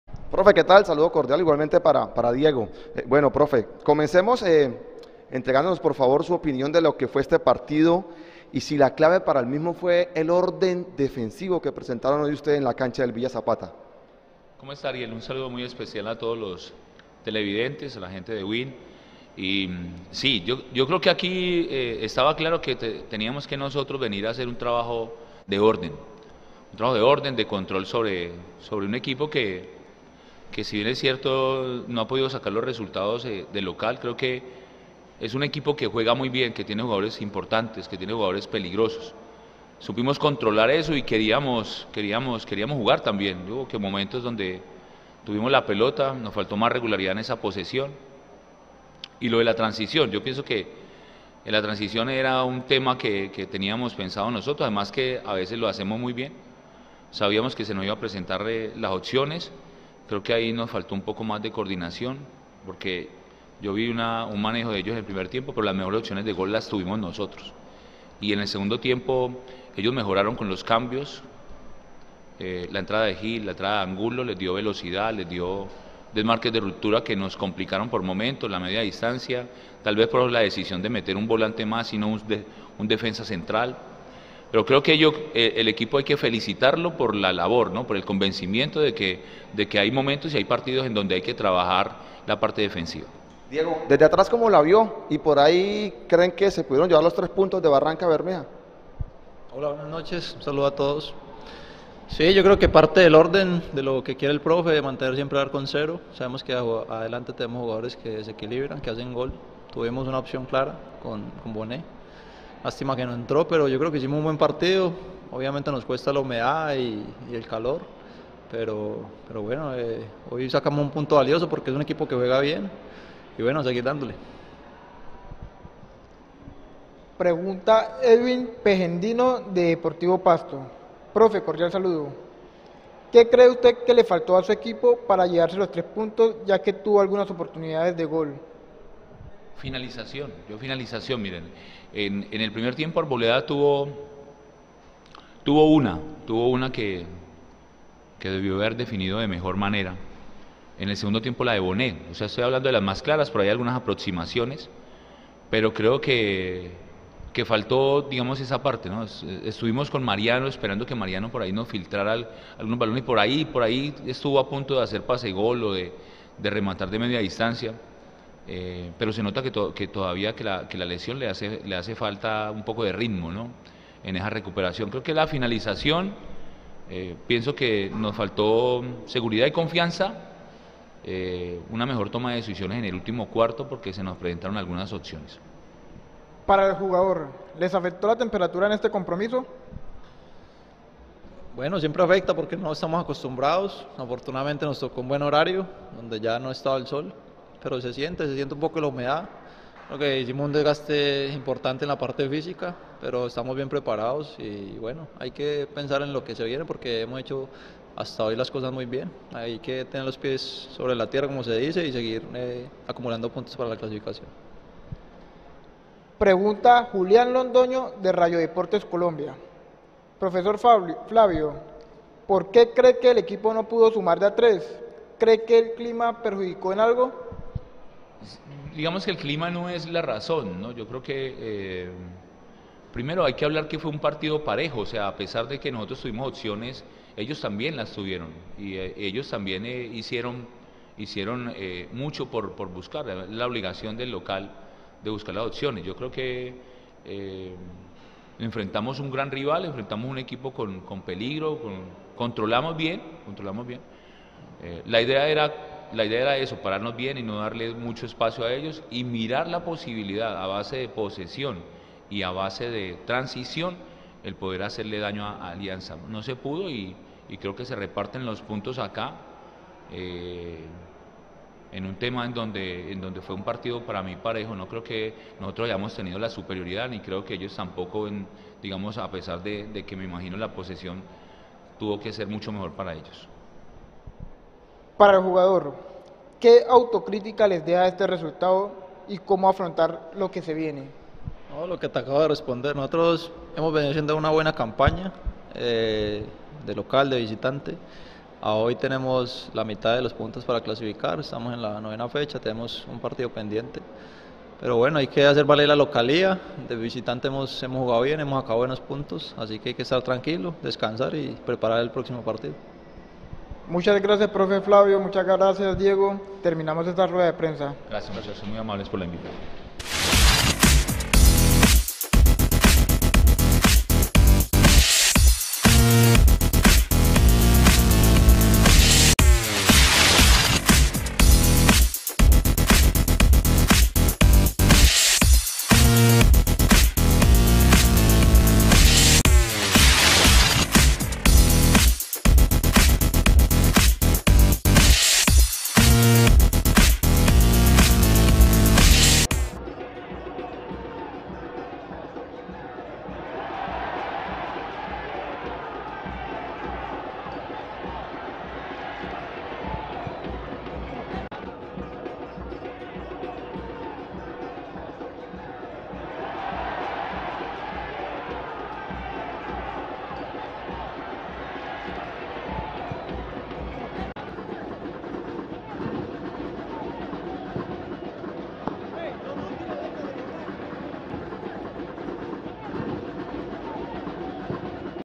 🔴🟡🔵CONFERENCIA DE PRENSA FECHA 9: FLABIO TORRES Y DIEGO MARTÍNEZ
🔴🟡🔵 A esta hora, recapitulamos las declaraciones de prensa del profesor Flabio Torres y del guardameta Diego Martínez tras el empate del volcán a domicilio ante Alianza Petrolera en auspicio de Aguardiente Nariño y Banco Av Villas.